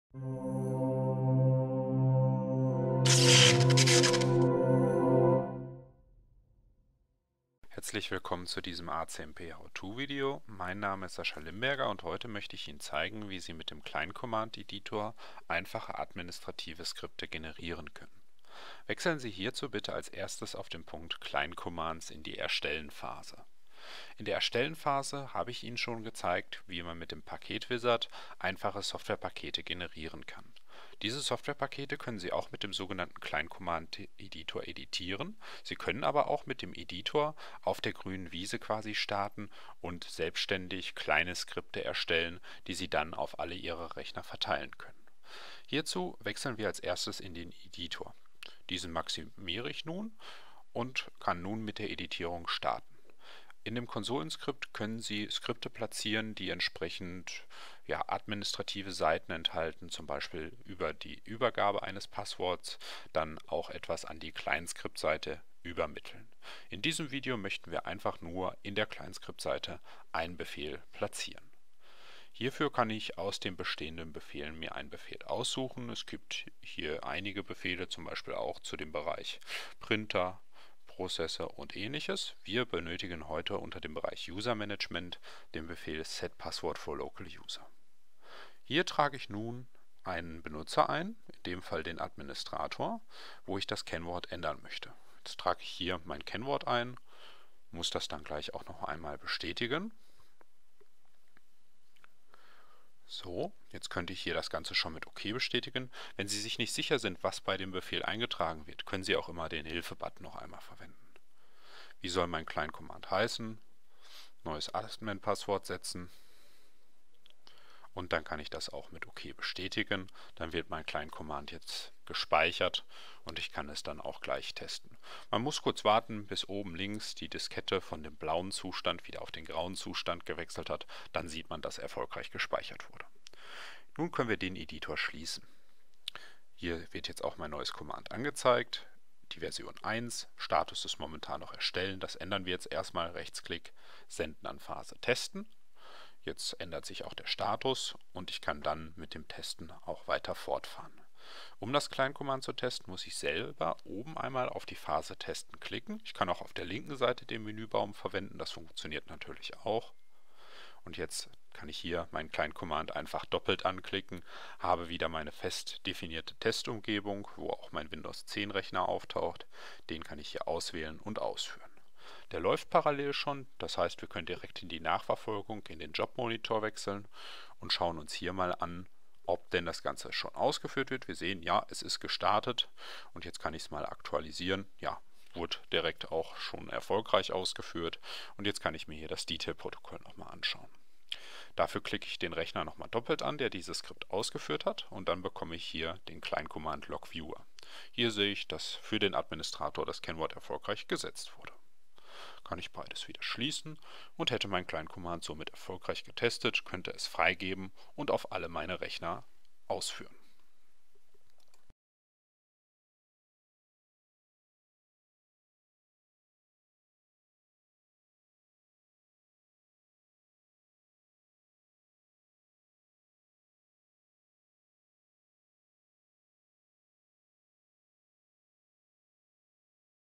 So erstellen Sie administrative Skripte mit dem Client Command Editor – ACMP Basics
Sie möchten in ACMP mit dem Client Command Editor einfach administrative Skripte erstellen? Wir zeigen Ihnen heute, was Sie beachten müssen.